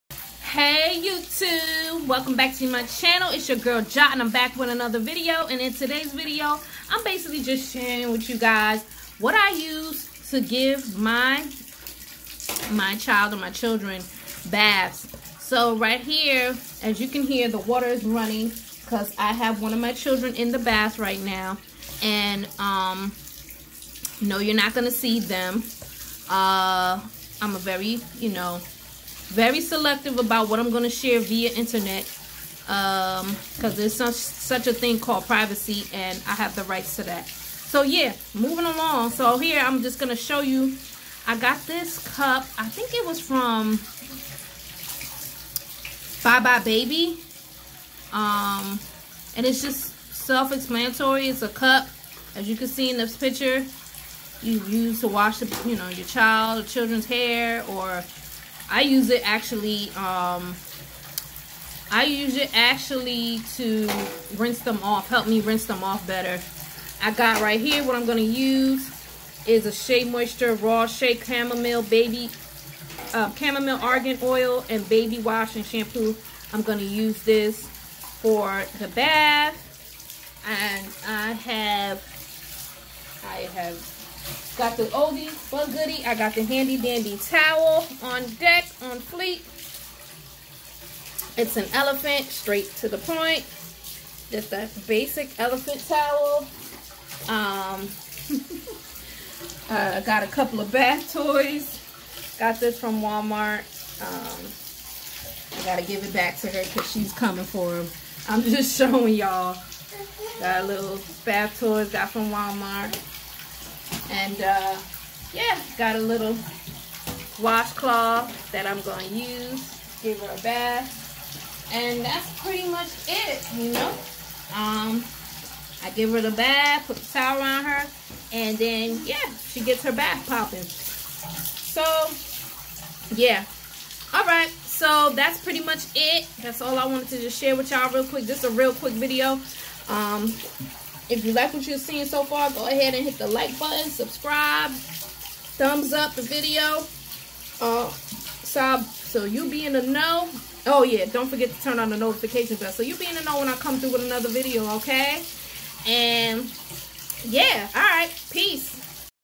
#41#bathtime Sheamoisture Walmart BuyBuyBaby BATHTIME ROUNTINE for Tots
Items I use to give my total a bath 🛁 💦🚿🧽🧼🚼